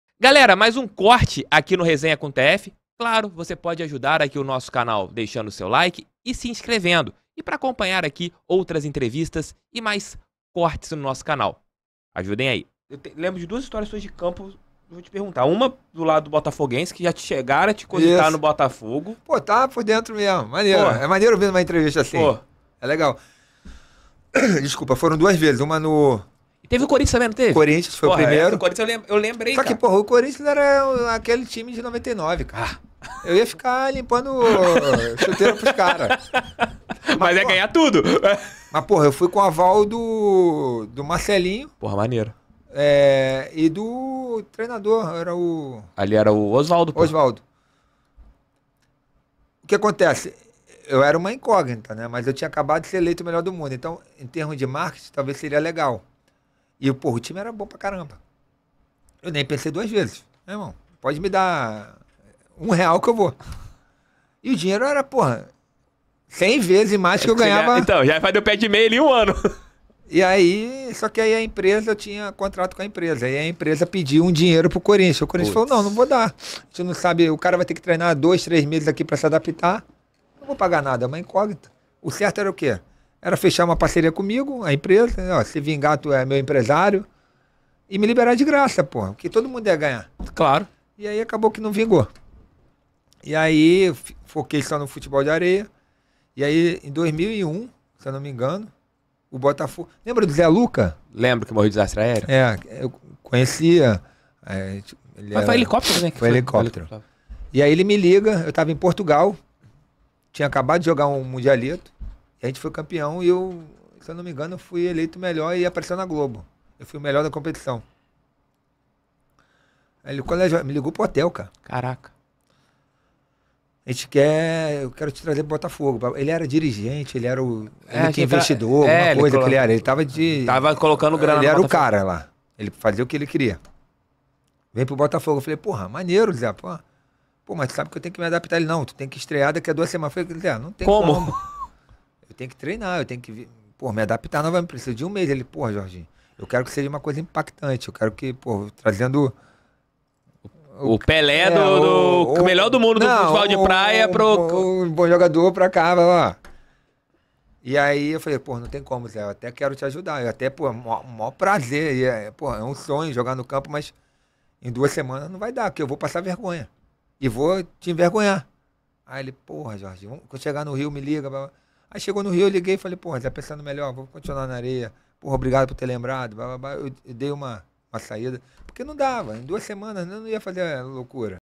BOTAFOGO E CORINTHIANS TENTARAM LEVAR JORGINHO DO BEACH SOCCER PARA OS GRAMADOS
Jorginho, um dos maiores jogadores da história do futebol de areia, conta que recebeu propostas do Corinthians em 99 e do Botafogo em 2001 e revela o porquê dos convites não terem ido mais adiante